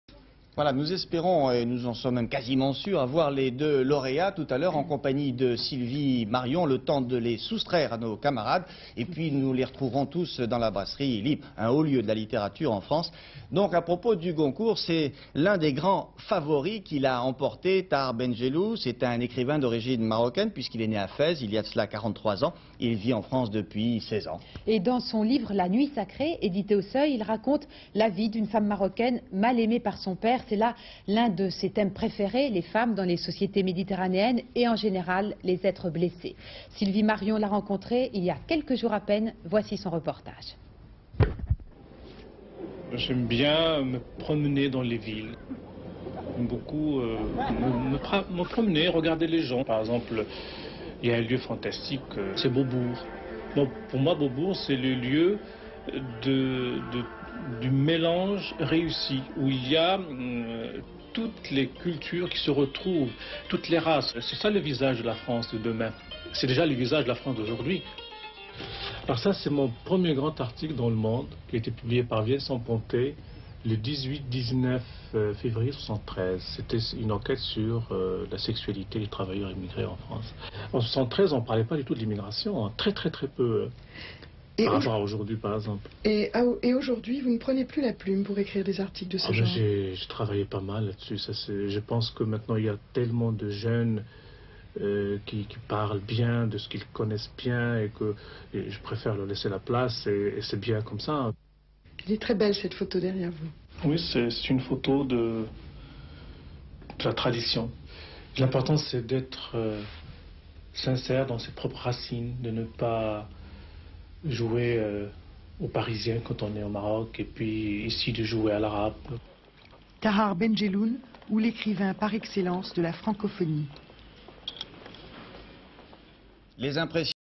Portrait Ben Jelloun
16 novembre 1987
Portrait de T BEN JELLOUN, prix Goncourt 1987 pr son livre "la nuit sacrée".      -                                                               celui-ci regardant des mimes sur l'esplanade du Centre Beaubourg ; interview off sur son intérêt pr le centre pompidou ; interview a son bureau ou il présente le 1er article qu'il a écrit pr le journal "le monde", consacre a la sexualité des immigrés; [gros plan] sa photo de mariage ; il prend un café a La Terrasse d'un bistrot. Images d'archive INA